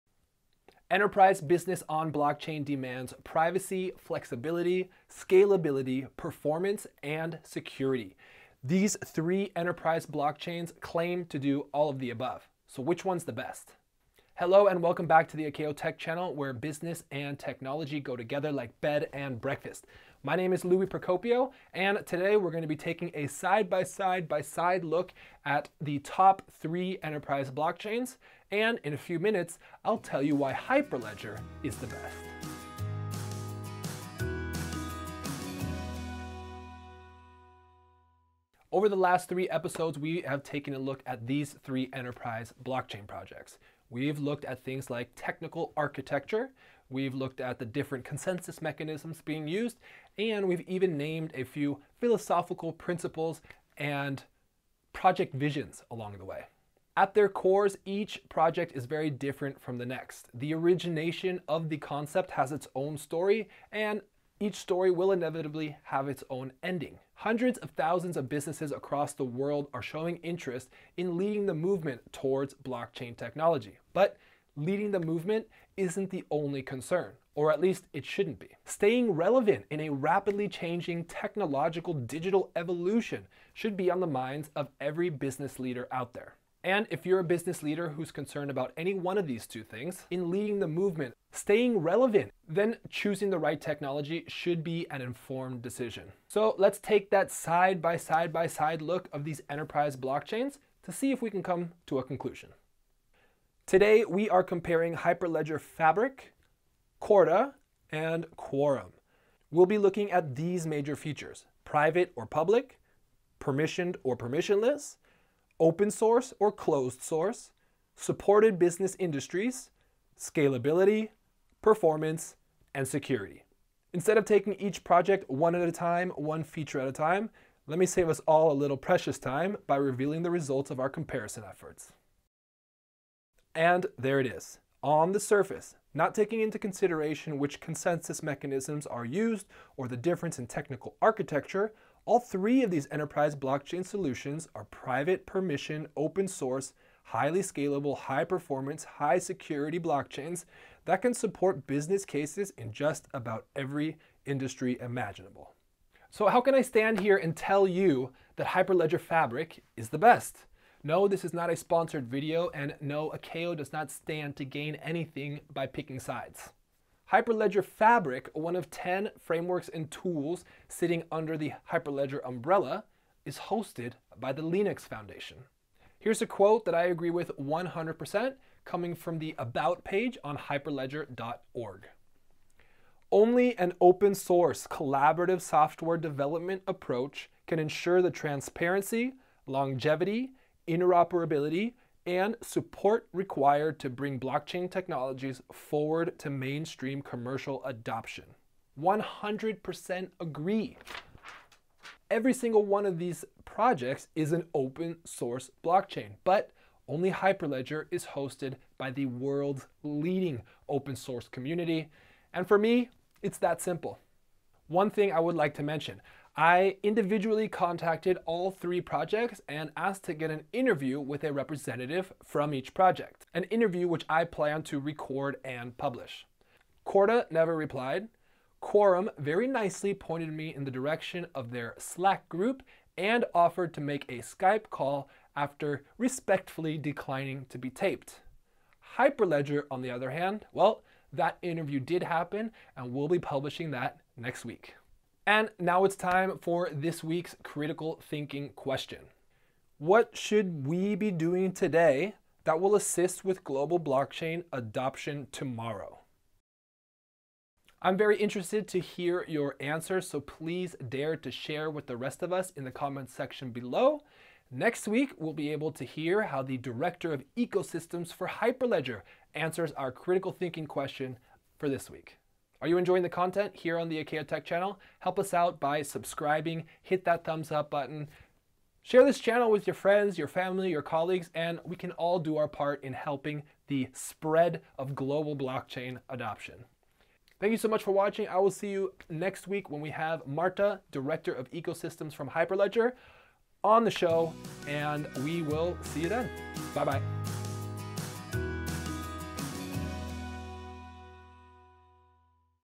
This is why your business should use Hyperledger Fabric | Top 3 private blockchain comparison
We've looked at HyperledgerFabric, Corda, and Quorum and compare all 3.

Just like there won't be a single cryptocurrency to rule them all, there won't be a single private blockchain either. But one of them has to be the best!

It was difficult for industries to adopt public, permissionless blockchains, the development of enterprise (private and permissioned) blockchains the adoption came easy. The industry giants started investing in the development of blockchain and even banking and financial sector got their eyes on this emerging technology. IBM and Linux Foundation collaborated and developed Hyperledger Fabric, R3 consortium consisting of banking giants developed Corda and JP Morgan designed and developed Corda. While all three are setting the platform for businesses to adopt blockchain and benefit from the technology, the likelihood of greater adoption lies with Hyperledger Fabric.

Queries Solved
1. The difference between Hyperledger Fabric, Corda and Quorum?
2. How enterprise blockchain is helping businesses to adopt blockchain technology?
3. Why we think Hyperledger Fabric is the best enterprise blockchain?

“Inspiring Change Through Technology.” – Akeo

If you wish to learn more about our services, please
or

We, at Akeo, make informative videos to help you learn everything about Blockchain technology.